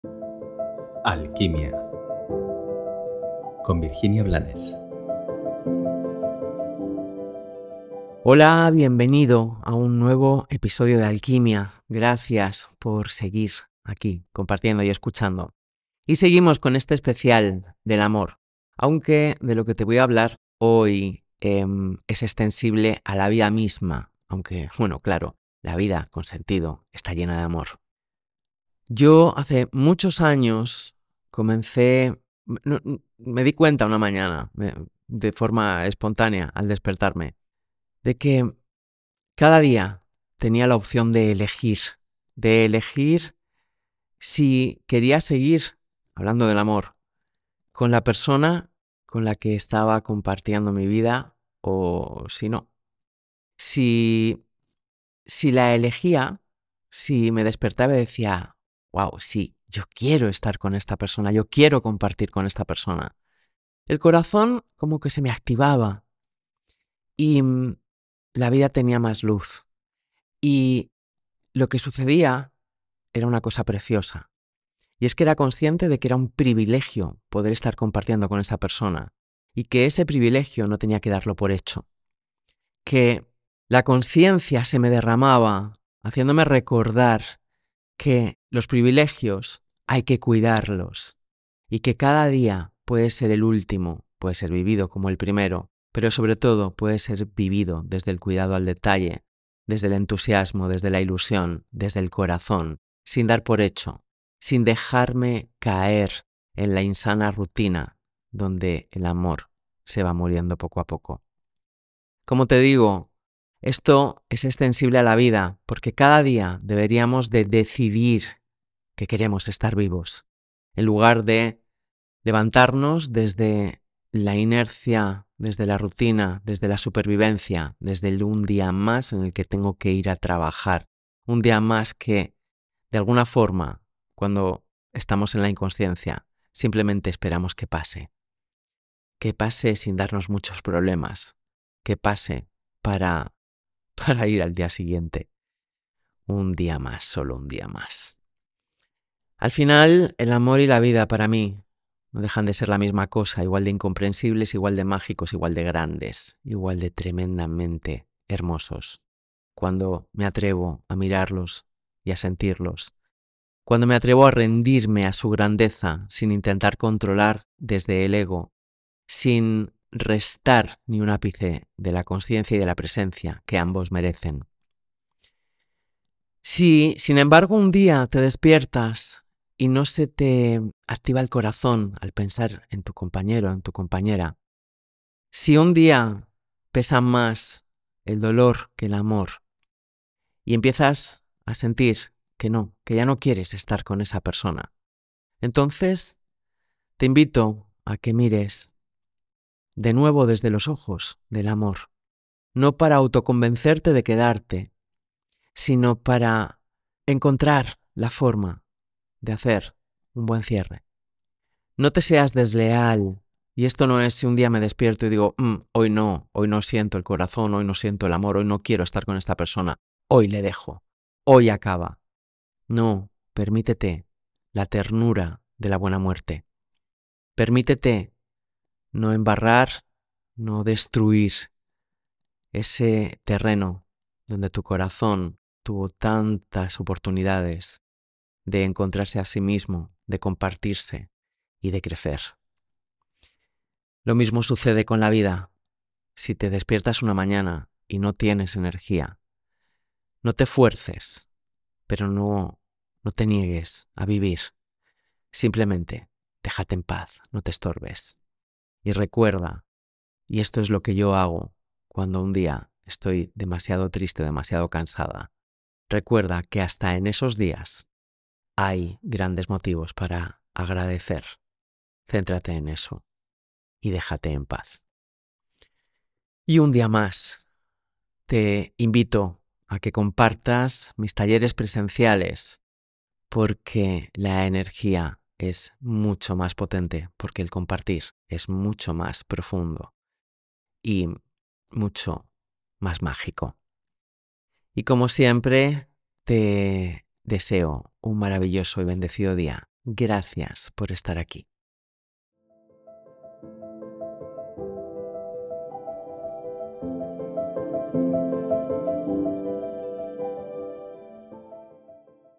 Cada mañana debo elegir. Episodio 223
Cuando das por sentados tus privilegios tu corazón se mustia y, sin darte cuenta, la rutina termina devorando la magia de la vida y del Amor.

¿Quieres saber más sobre mi trabajo? Sígueme